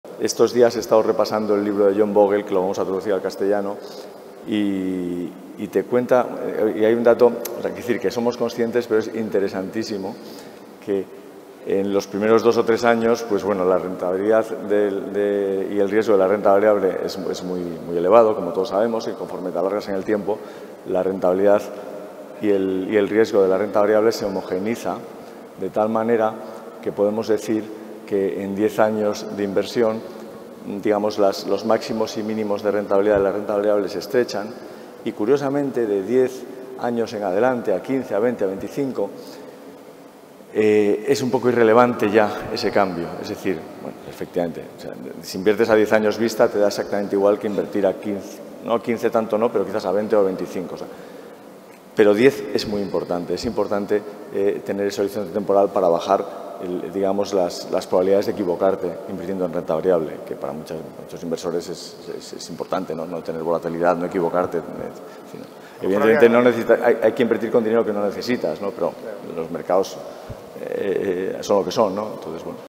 Fragmento de Entrevista Iberian Value a Francisco García Paramés
García Paramés nos cuenta cómo la rentabilidad y el riesgo de la renta variable se homogeinizan con el tiempo y resalta la importancia del largo plazo en la inversión.

El libro de John Bogle, “Cómo invertir en fondos de inversión con sentido común”, recientemente ha sido traducido por Value School.

Entrevista realizada por Alberto Artero Salvador el pasado mes de julio.